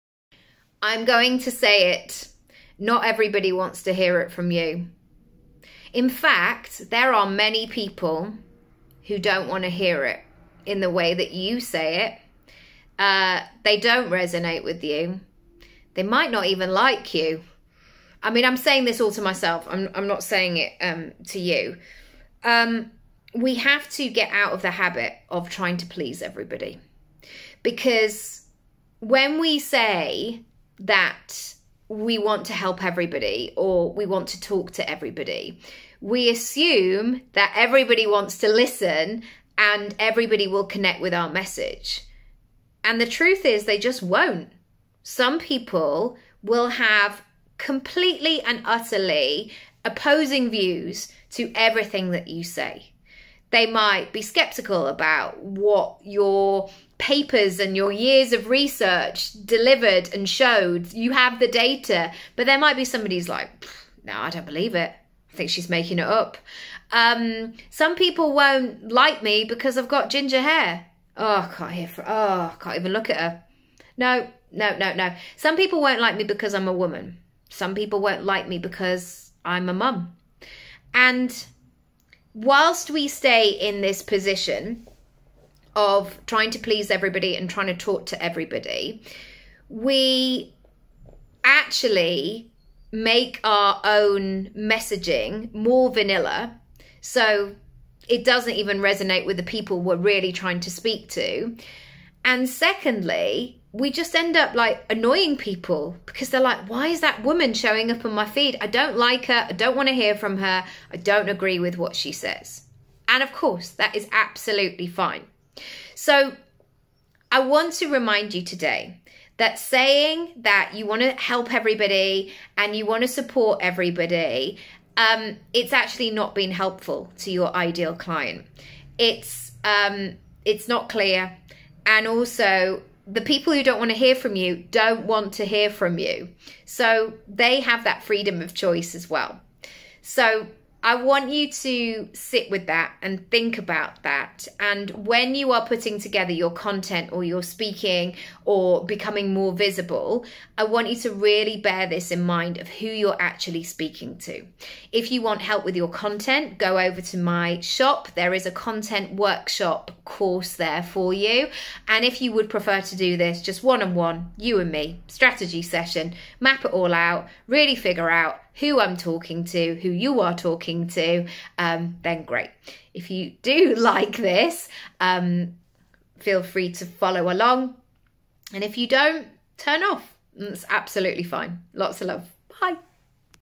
Not everyone wants to hear it from you and that works well for everyone! Nicky Raby
We think that wanting to help everyone with our business is the kindest thing we can do...

However there will be many people who won't want to hear it from you. They won't resonate. They won't connect with you. They may just prefer another alternative.

So knowing that information gives you full permission to be you. That way, the people who want to hear from you, learn from you, connect with you, buy from you - can. And then you don't have to bother the others who want to explore a more suited option..